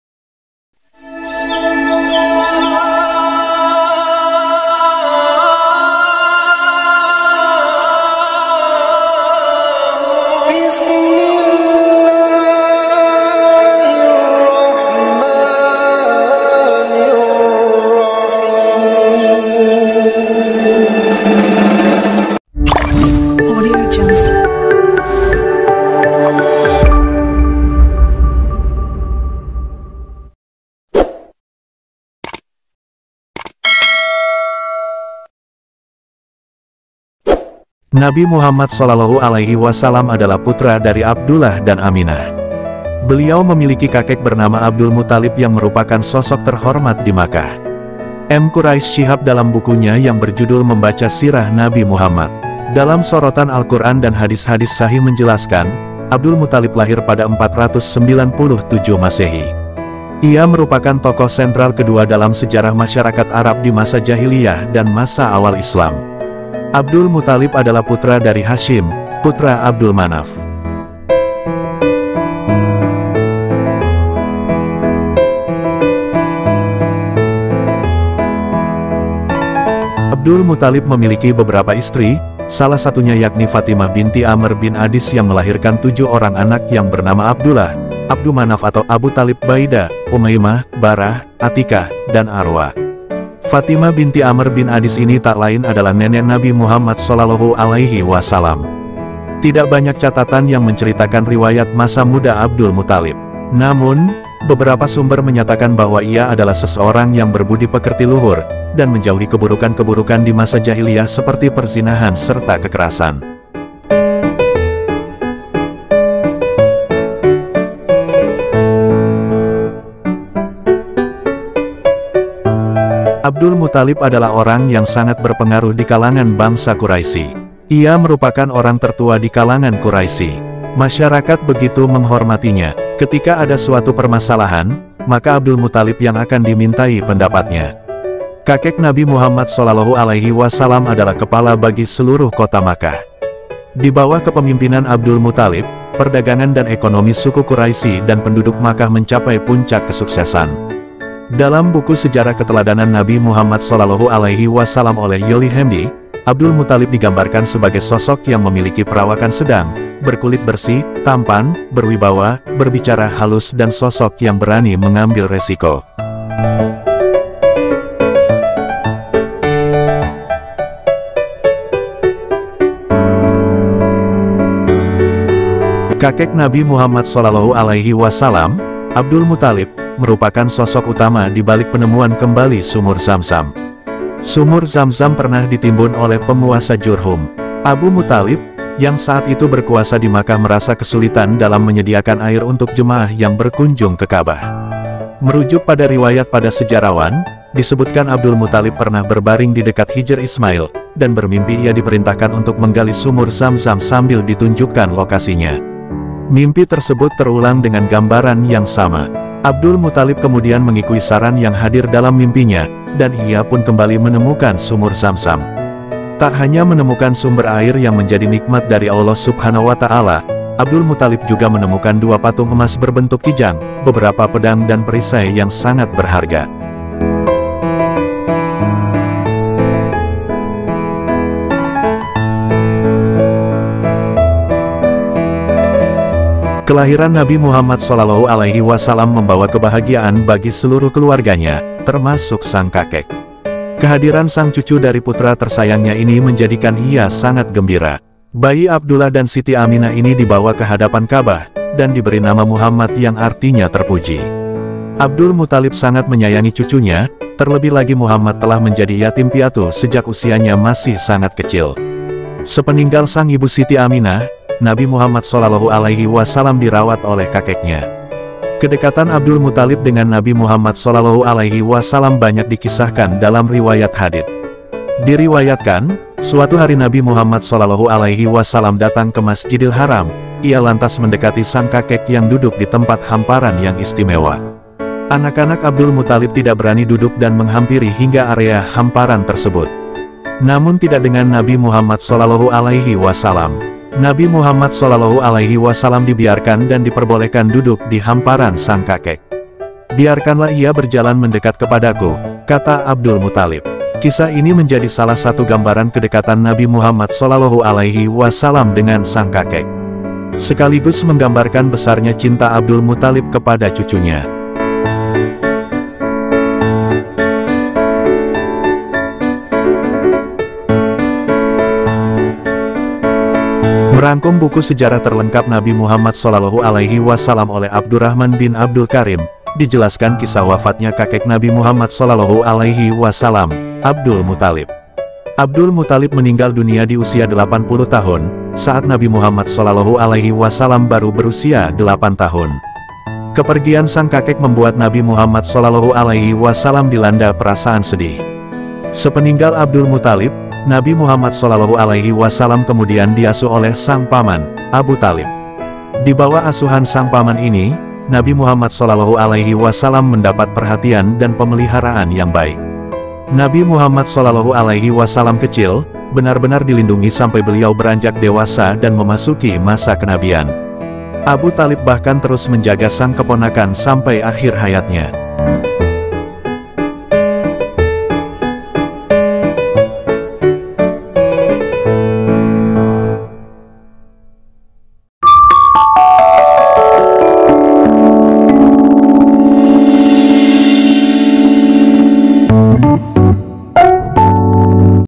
Mengenal Kakek Nabi Muhammad SAW dari Nasab Ayahnya
Nabi Muhammad SAW adalah putra dari Abdullah dan Aminah. Beliau memiliki kakek bernama Abdul Muthalib yang merupakan sosok terhormat di Makkah.